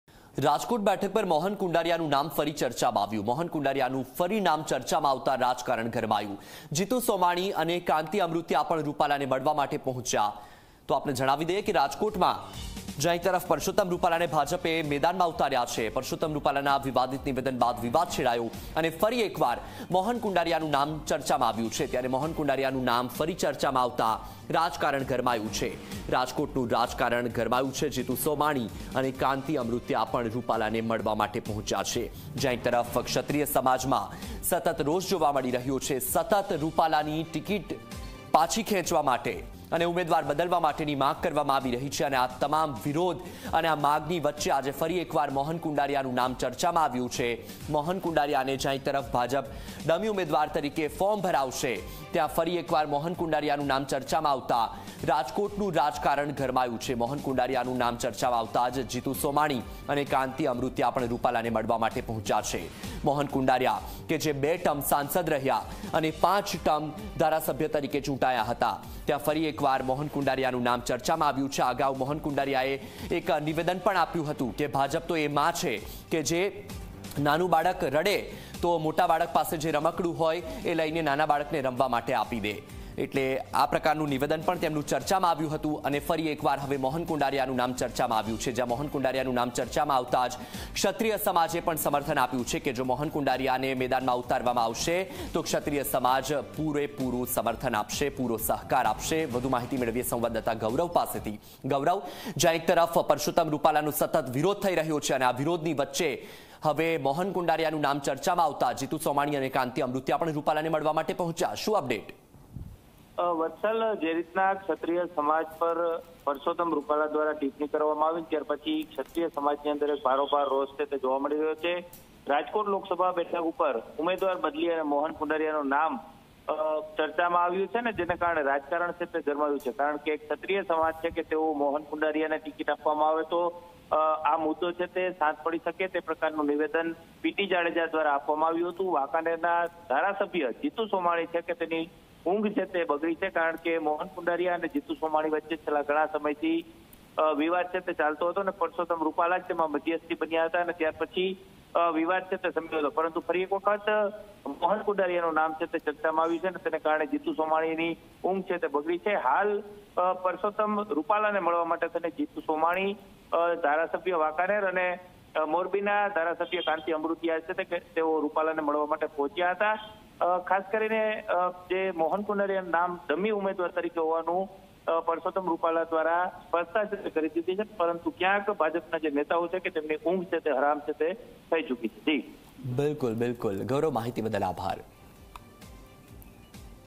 Amid speculations of BJP replacing candidate on Rajkot seat, Jitu Somani, Kanti Amrutiya meet Rupala
Amid speculations of BJP replacing candidate on Rajkot LS seat, Jitu Somani, Kanti Amrutiya arrive to meet Rupala

રાજકોટ બેઠક પર મોહન કુંડારિયાનું નામ ફરી ચર્ચામાં   ---

મોહન કુંડારિયાનુ ફરી નામ ચર્ચામાં આવતા રાજકારણ ગરમાયું  ---

જિતુ સોમાણી અને કાંતિ અમૃતિયા પણ રૂપાલાને મળવા પહોંચ્યા